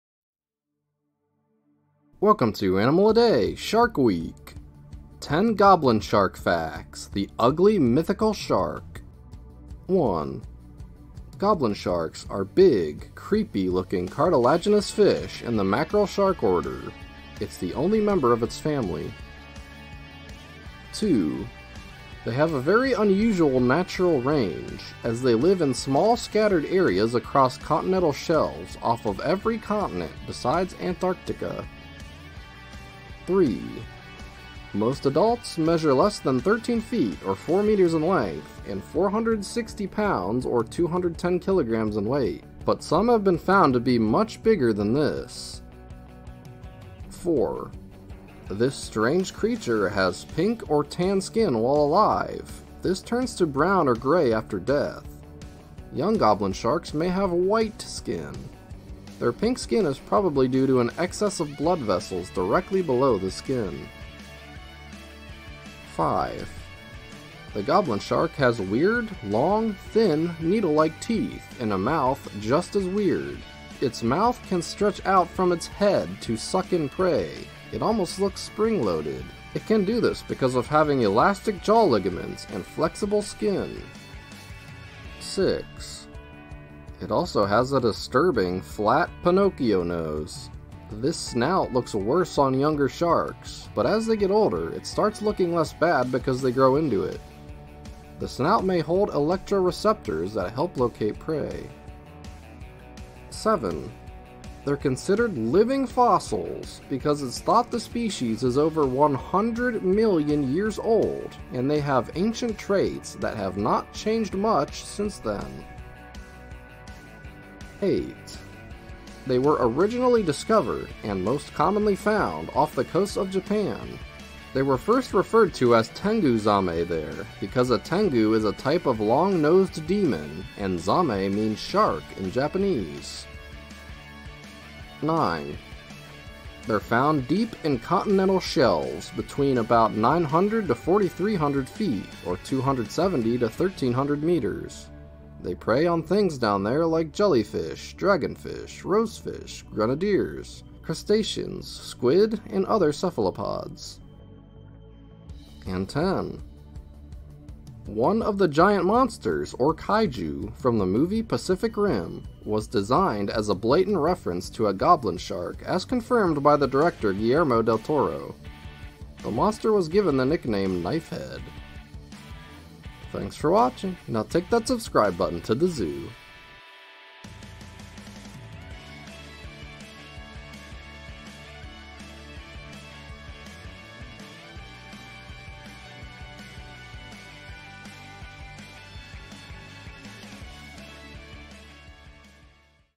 Goblin Shark Facts - The Ugly Mythical Shark - Animal a Day
Welcome to Shark Week! This week we will be talking about a different shark species every day. Today we learn about the horrific looking monster of the deep, that literally throws its mouth at prey, the Goblin Shark.